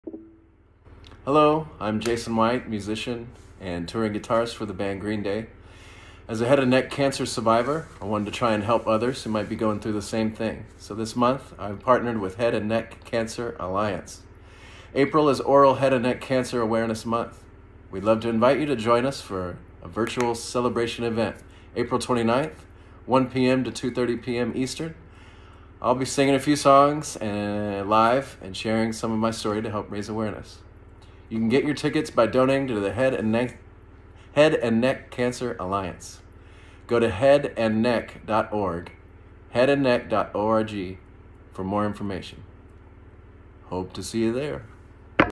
Jason White of Green Day invites you to join the Move-A-Thon Celebration
Join HNCA for a FREE Move-A-Thon celebration event featuring Green Day's Jason White.  Jason, a survivor of tonsil cancer, will be performing several of his signature songs (live on Zoom) to thank the many 2023 Move-A-Thon participants and honor all patients and survivors of head and neck cancers. Additionally, Jason will be discussing his cancer journey with his oncologist and HNCA board member Dr. Sue Yom.

This event is free to attend but does require registration/RSVP.